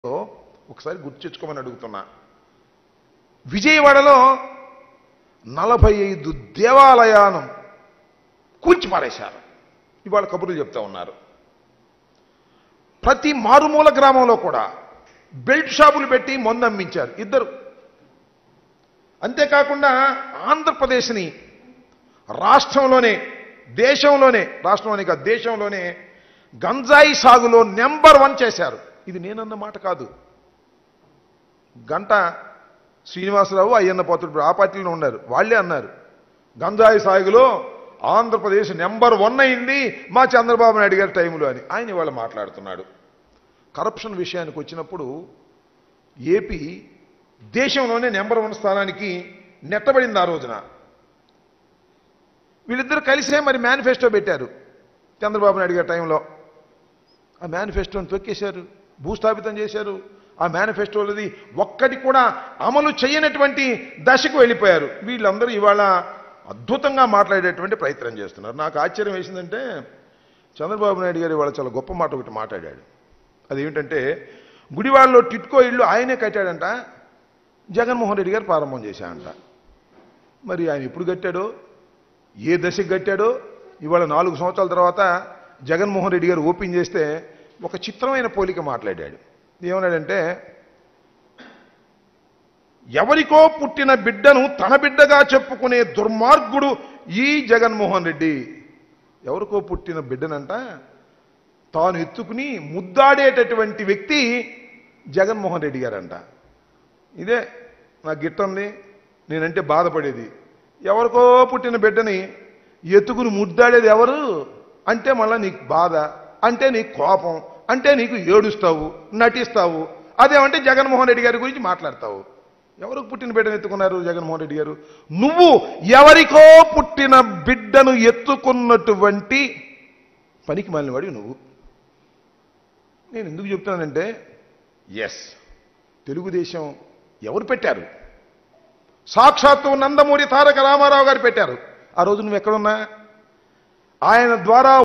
దేశంలోనే గంజాయి సాగులో నెంబర్1 అయింది..! | Ambati Ramababu Fires on Chandrababu
దేశంలోనే గంజాయి సాగులో నెంబర్1 అయింది..! | Ambati Ramababu Fires on Chandrababu | Hit TV Telugu News